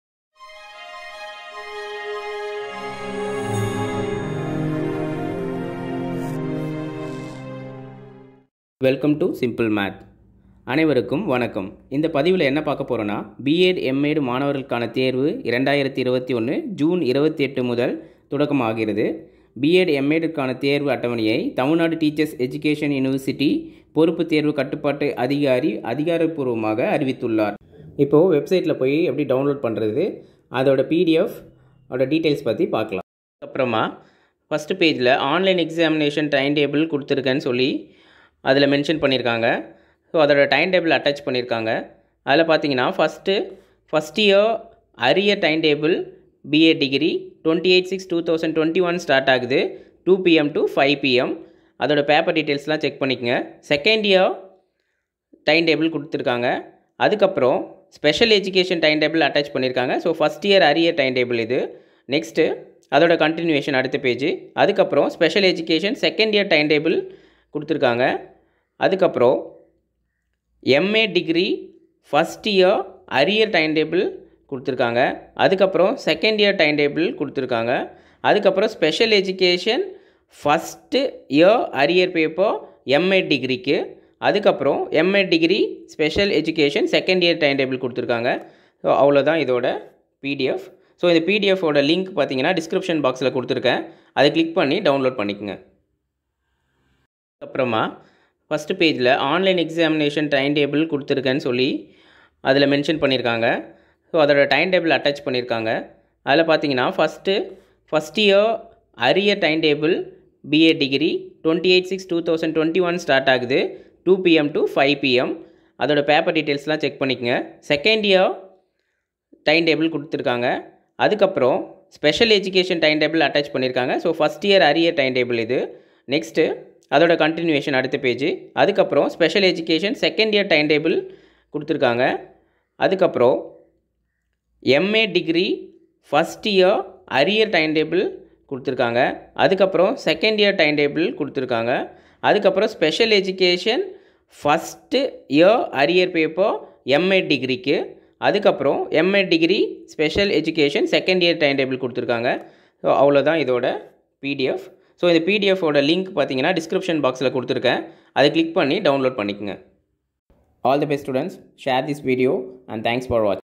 TAMILNADU| TNTEU| B. ED.,| M. ED., TIME TABLE |ANNOUNCED| ONLINE| EXAM| 28.6.2021| பிஎட்,எம்எட்
பிஎட் எம்எட் காண தேர்வு அட்டவணை தமிழ்நாடு TNTEU வெளியீடு